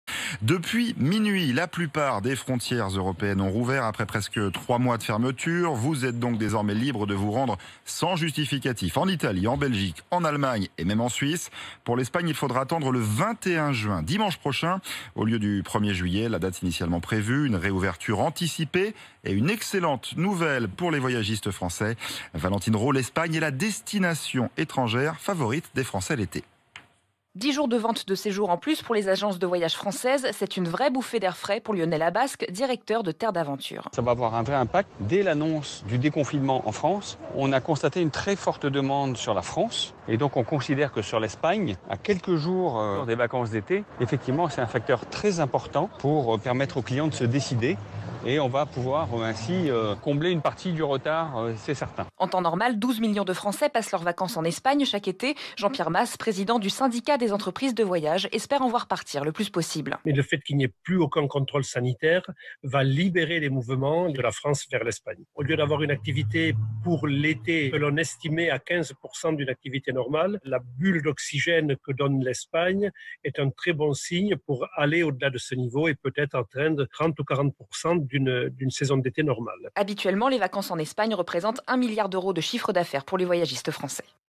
Réouverture anticipée des frontières: une excellente nouvelle pour les voyagistes français
Depuis minuit ce lundi matin, la plupart des frontières européennes ont rouvert après presque 3 mois de fermeture. L'Espagne est la destination étrangère favorite des Français l'été